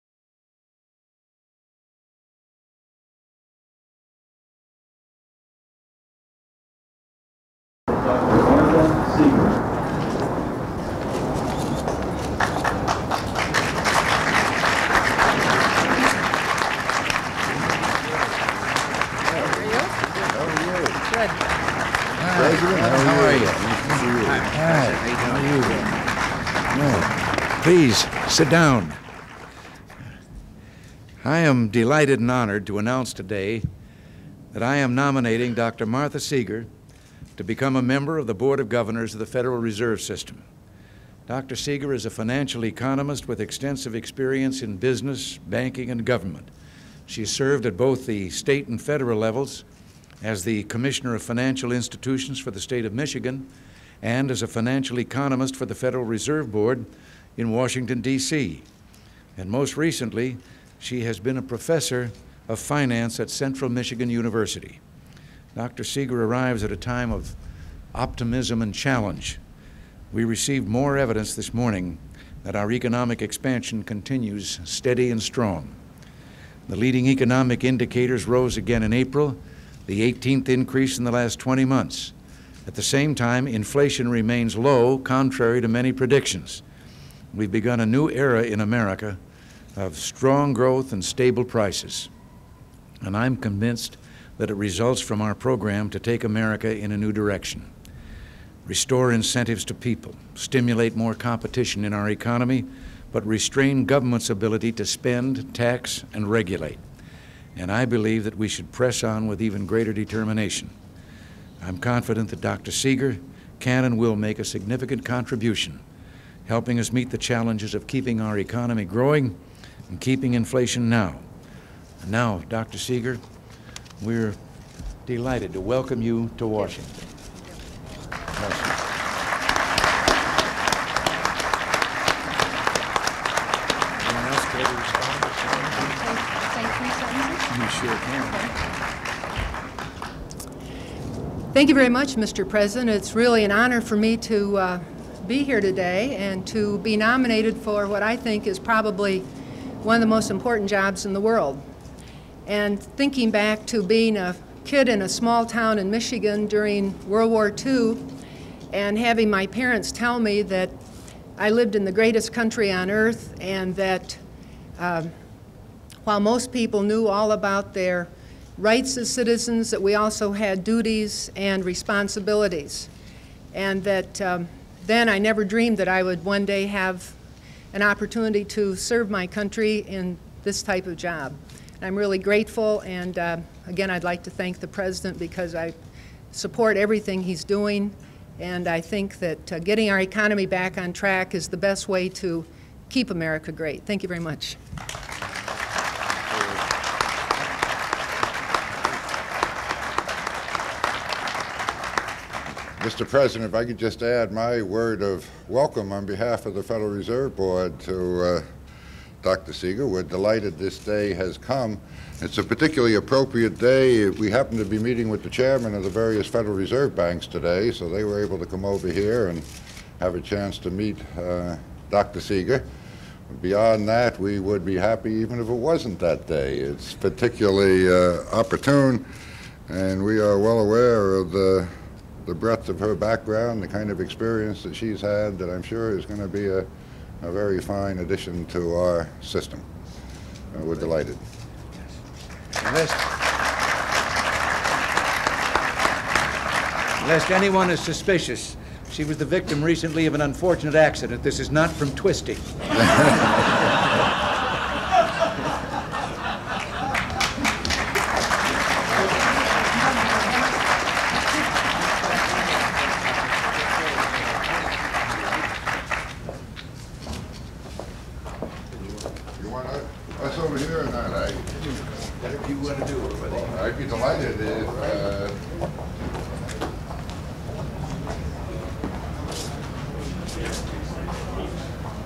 President Reagan's Remarks Announcing the Nomination of Martha R. Seger on May 31, 1984
Full Title: Remarks Announcing the Nomination of Martha R. Seger To Be a Member of the Board of Governors of the Federal Reserve System in Rose Garden on May 31, 1984

Access Restriction(s):Unrestricted
Use Restriction(s):Unrestricted